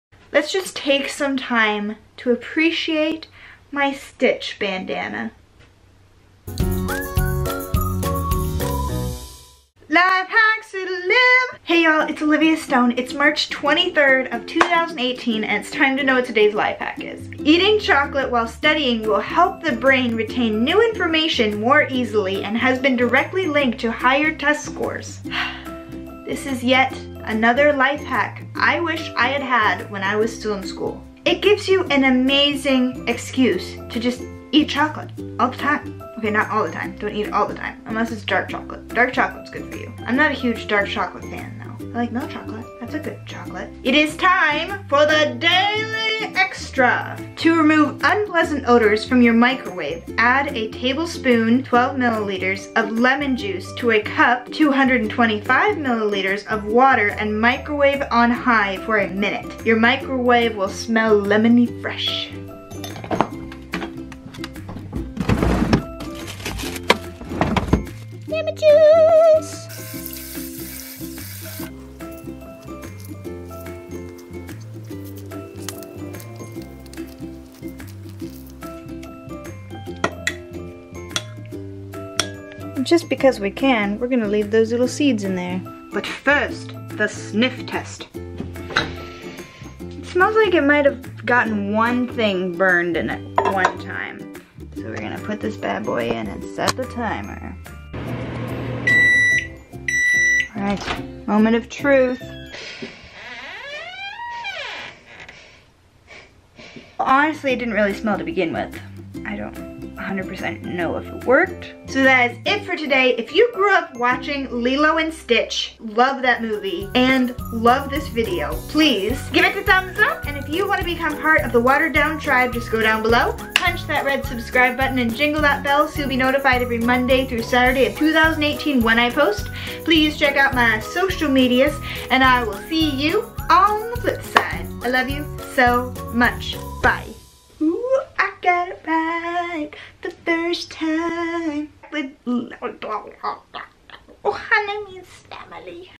Chocolate makes you Smarter | Life Hacks with Liv
There is no more need for you to feel self conscious eating chocolate anymore! Phew! Thank goodness!